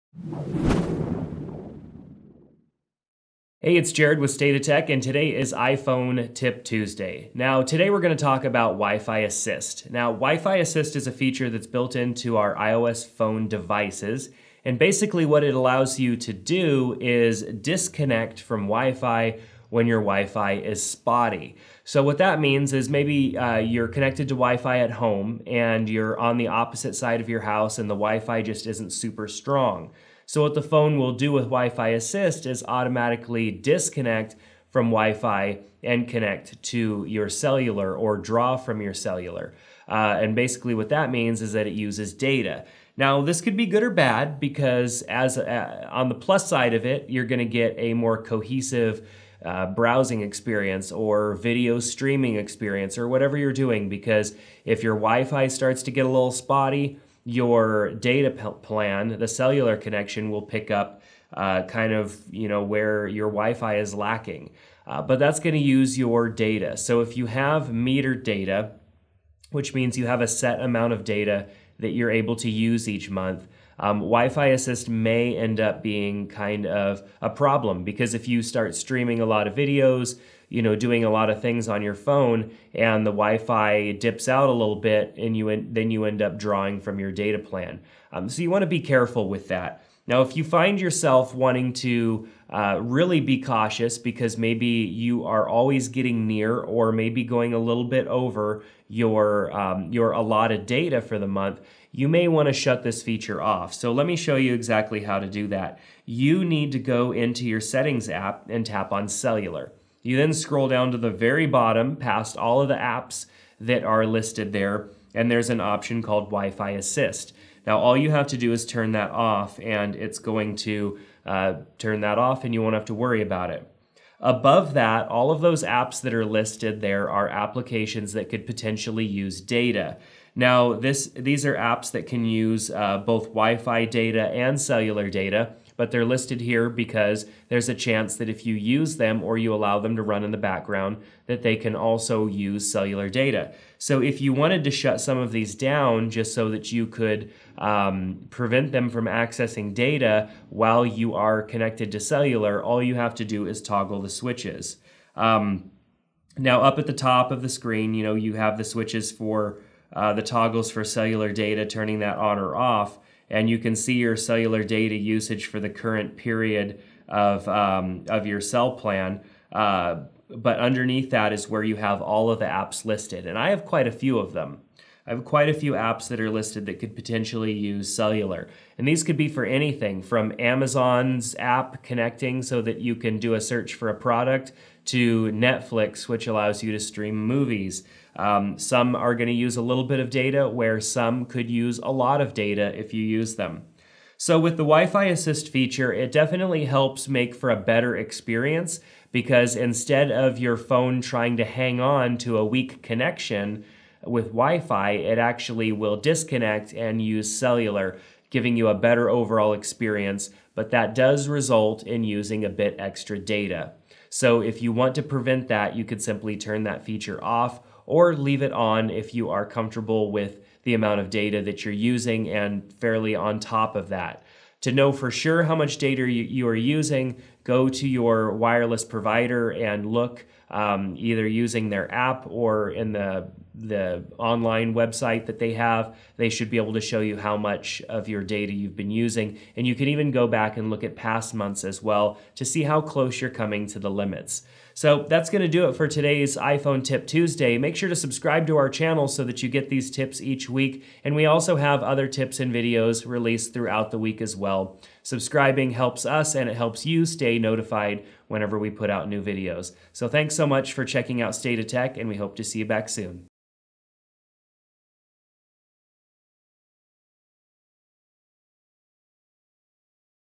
How to Avoid High Data Usage on iPhone's with iOS 9
If you feel that you've been using more data than usual, chances are you may be right. The WiFi Assist feature on the iPhone helps keep your internet connection as strong as possible, even if that means ditching WiFi! Check out how to use this feature on today's iPhone Tip Tuesday.